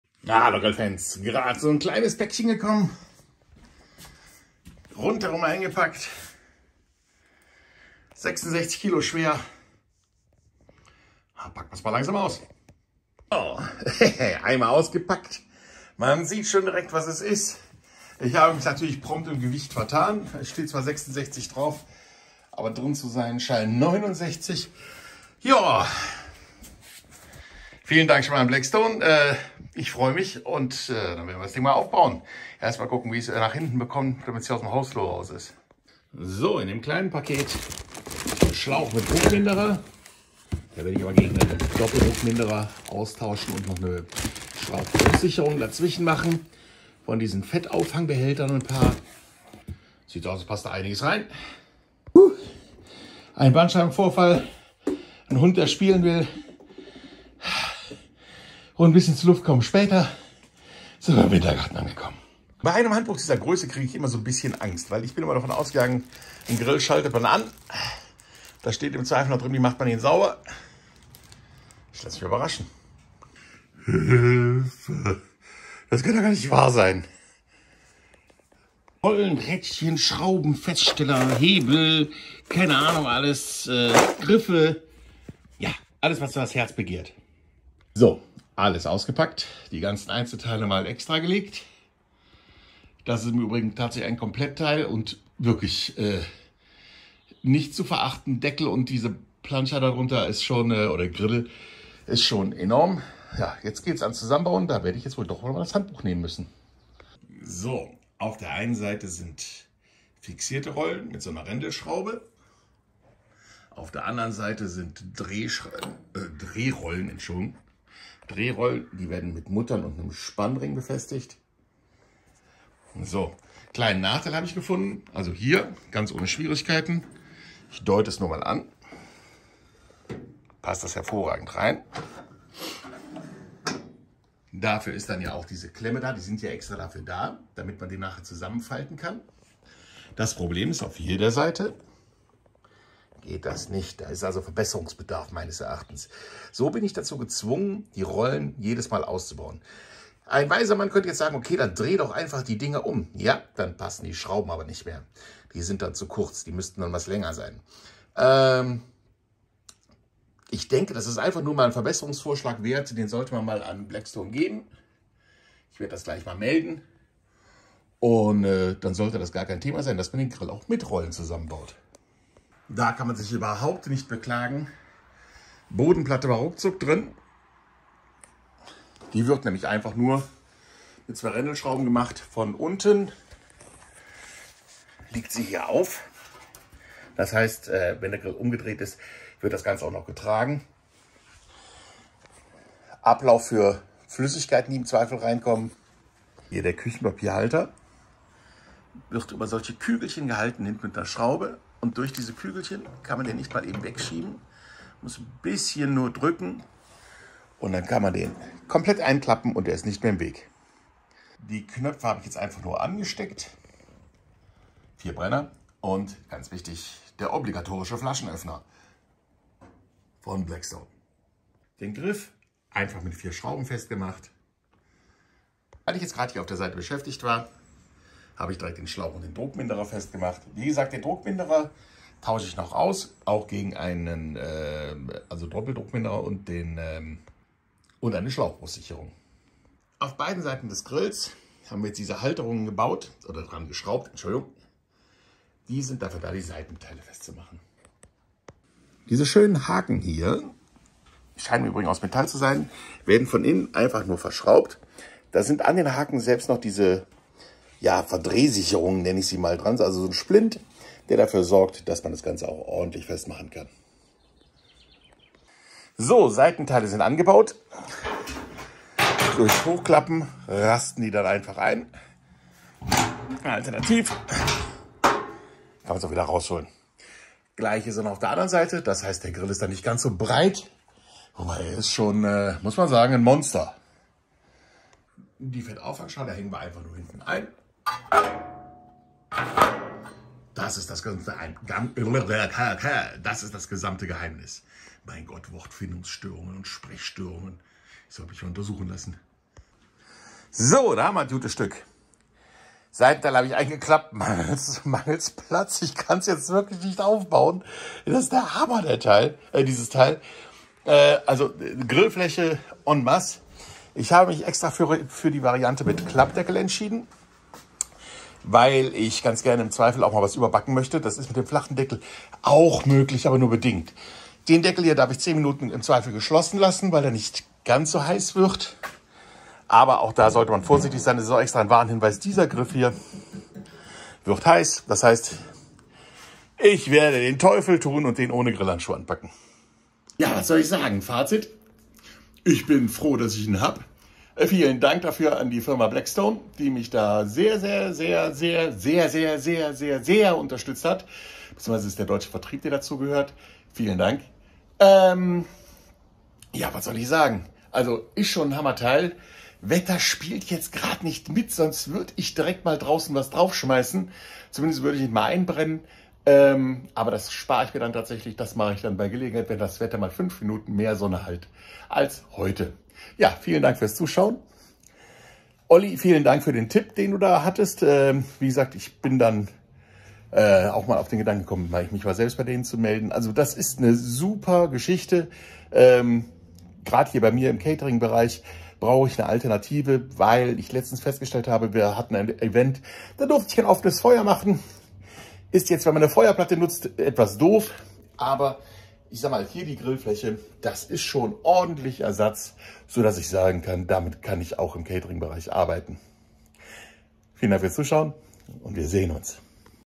Grillkanal - Blackstone Griddle - Unboxing und Aufbau
Hallo Grillfans!

willkommen auf meinem Grillkanal

Sascha